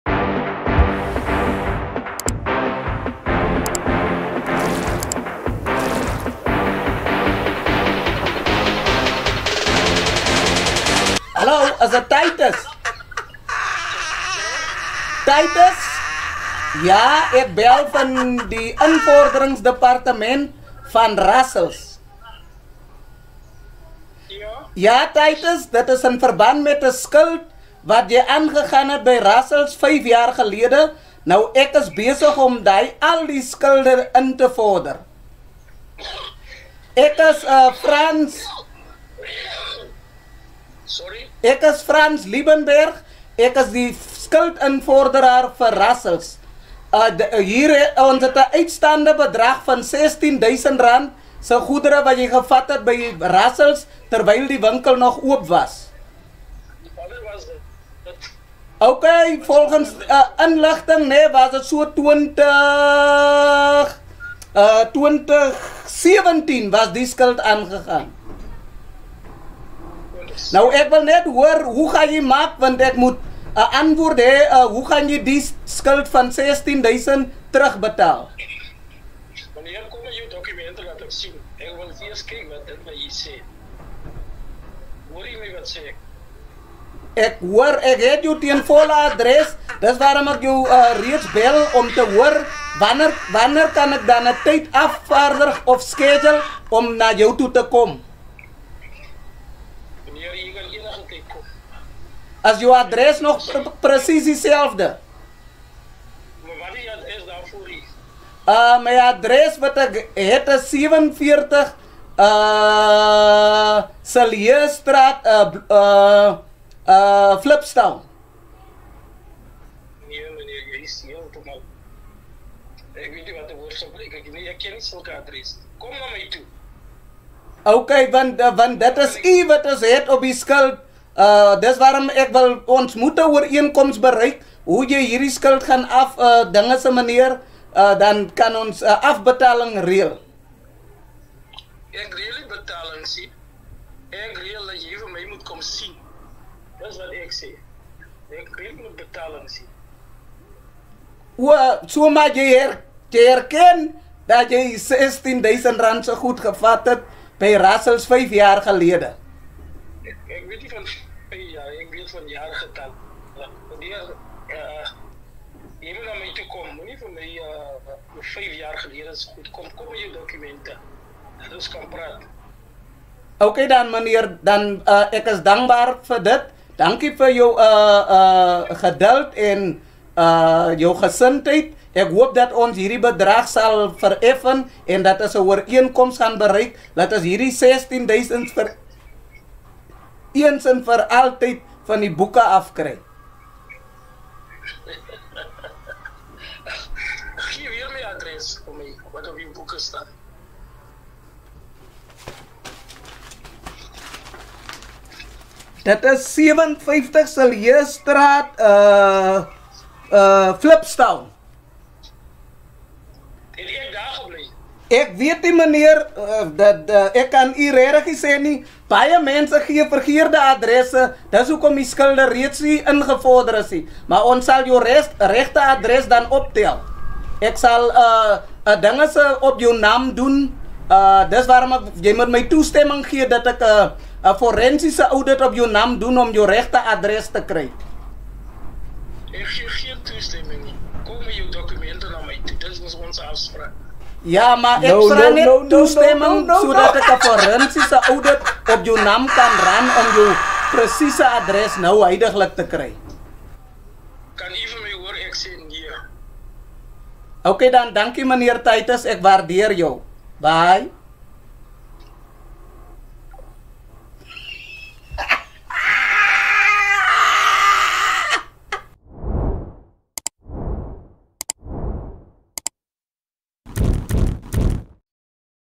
FAKE OUTSTANDING BALANCE PRANK
PRANKING PEOPLE TELLING THEM THEY STILL OWE ON THEIR ACCOUNTS